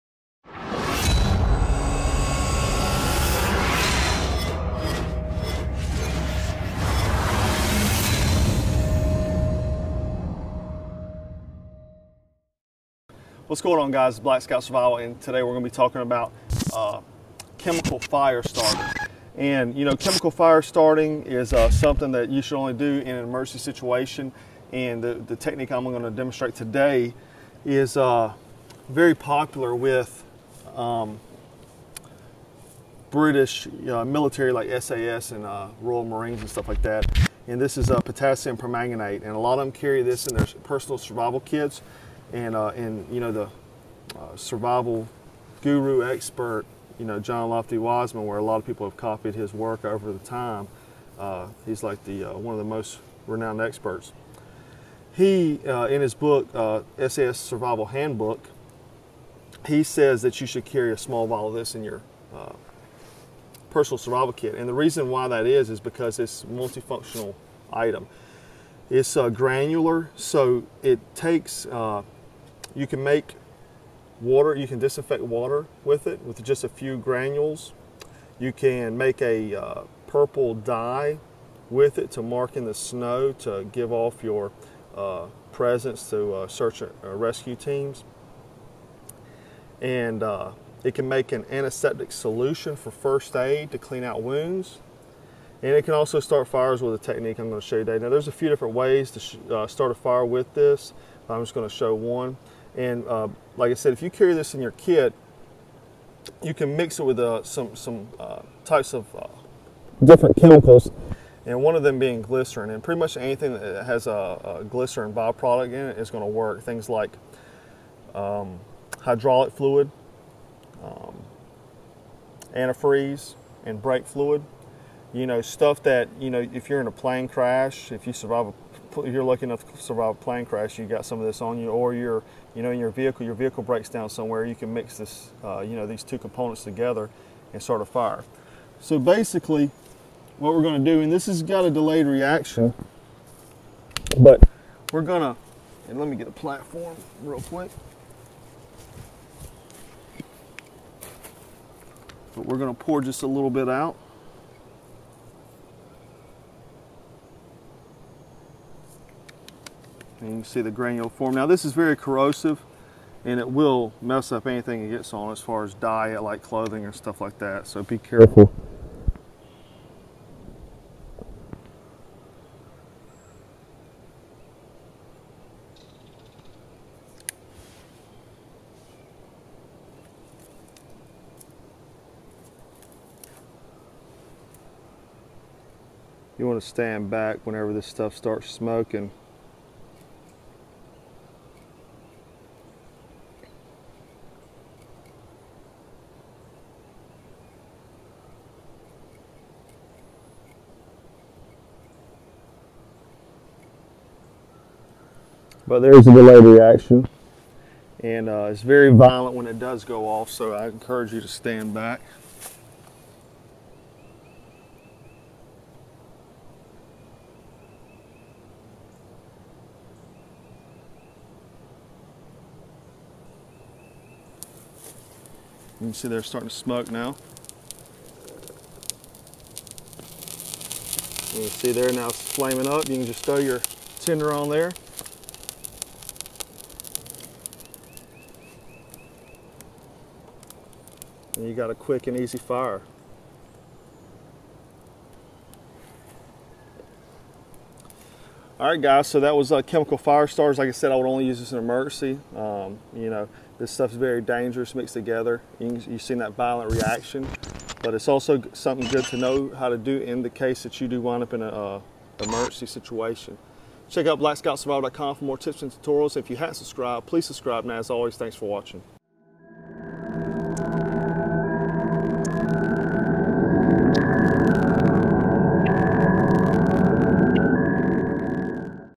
How to Start a Chemical Fire- Black Scout Tutorials
This video is about Chemical Fire Starting using some common items. This is a last ditch method that could save your life. I recommend only doing this in the case your life is on the line.